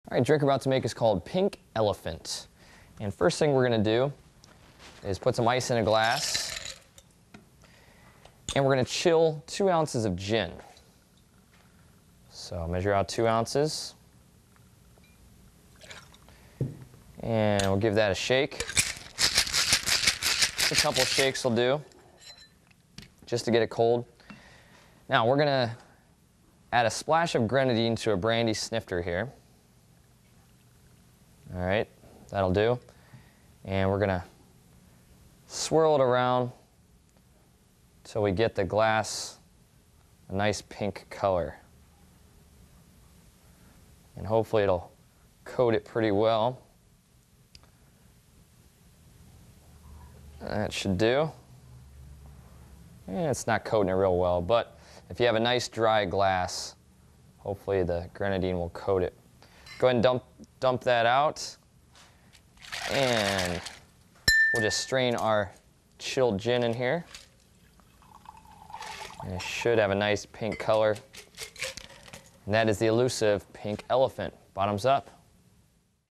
Gin Mixed Drinks: Part 3 : How to Make the Pink Elephant Mixed Drink
The Pink Elephant is a mixed drink with gin that's easy to make at home. Our professional bartender shows you how in this free video on mixed drinks.

Expert: Expert Village Bartender
Bio: Our Expert Village bartender has over 10 years of experience concocting drinks.